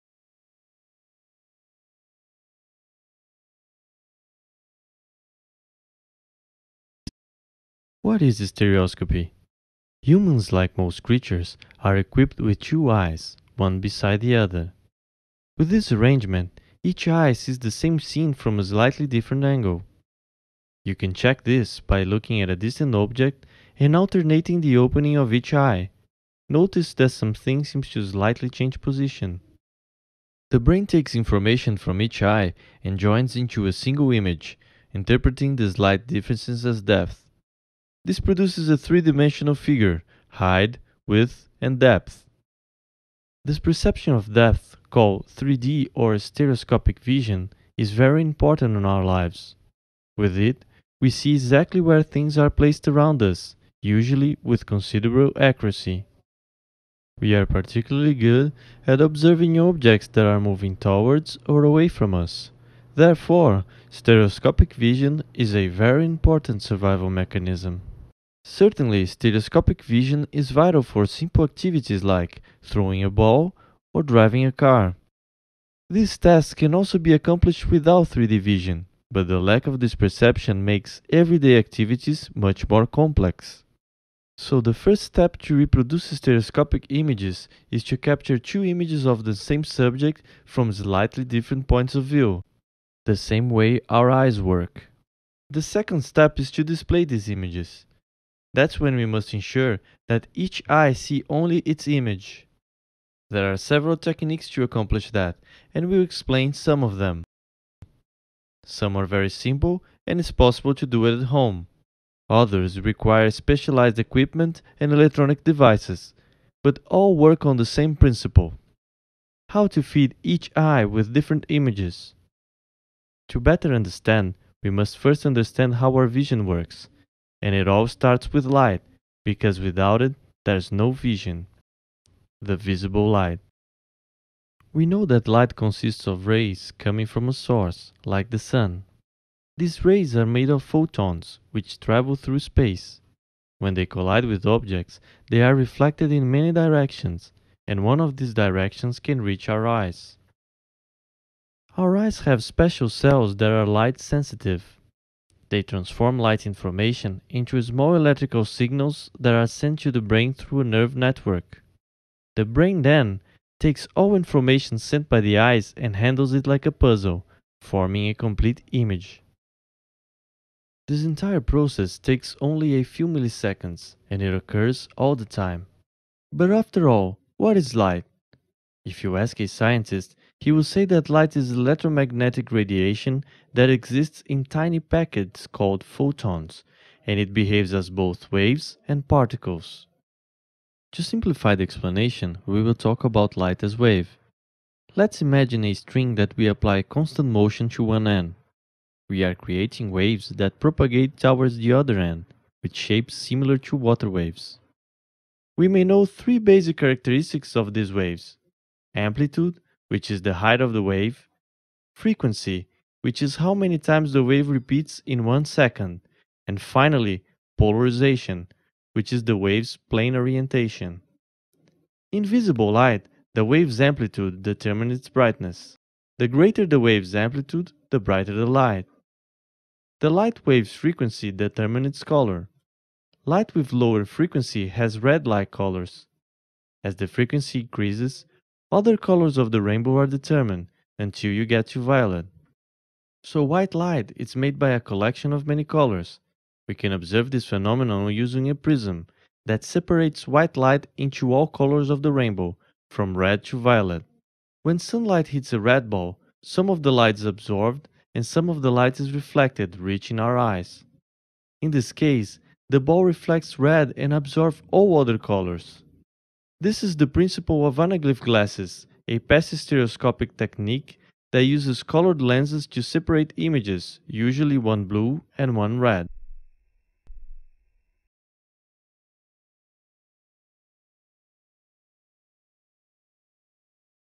What Is Stereoscopy?
Este video foi desenvolvido pela Caverna Digital, o laboratório de Realidade Virtual da Universidade de São Paulo, para explicar o que é Estereoscopia. Faz parte de uma série cujo objetivo é explicar os conceitos básicos de Realidade Virtual para os visitantes de uma maneira simples e divertida. Foram utilizadas técnicas de animação digital, predominantemente em estilo cut-out.

Category: Computer Animation Shorts